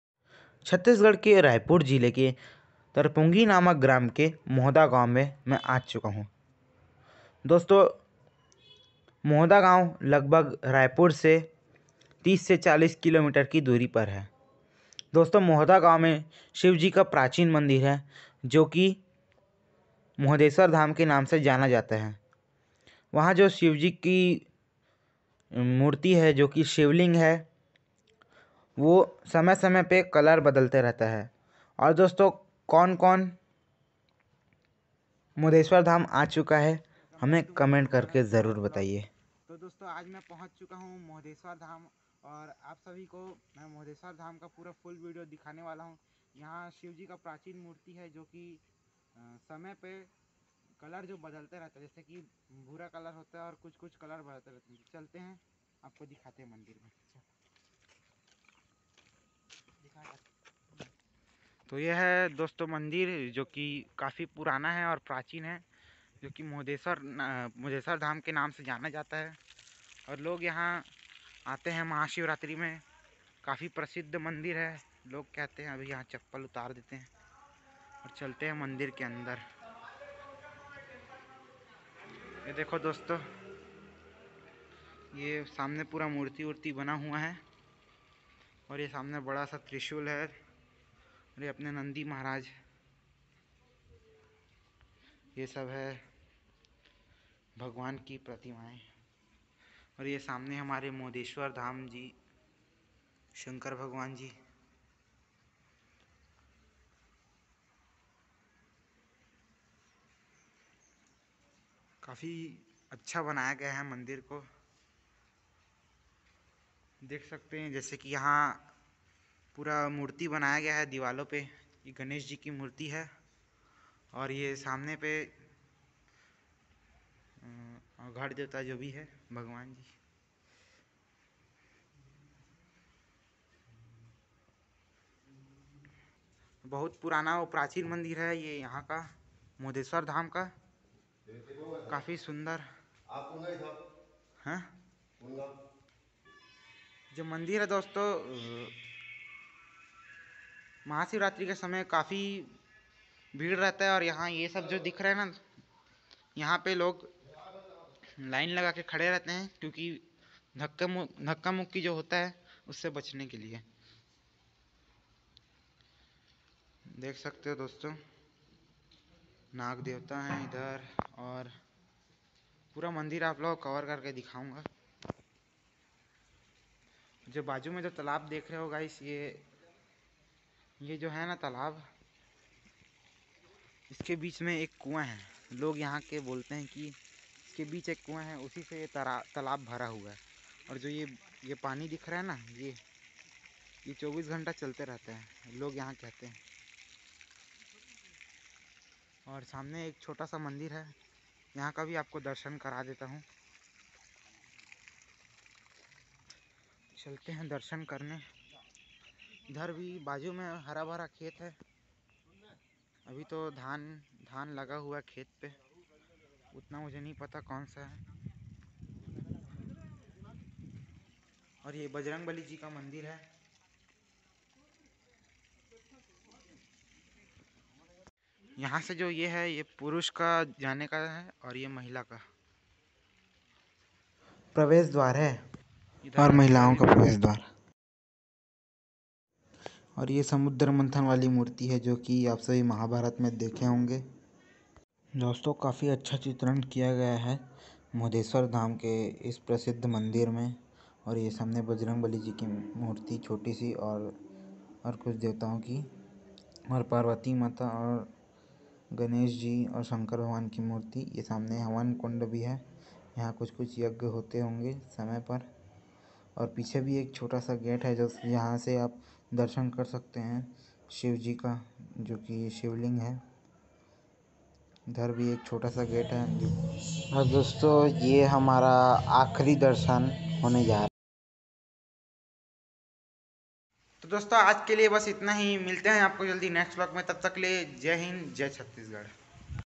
Tarpongi gram ke Mohda gaon ka prachin or prasiddh mandir Mohdeshvardham🚩 C. G
आप सभी को मेरे तरफ से महाशिवरात्रि की हार्दिक शुभकामनाय हर हर महादेव 🚩🚩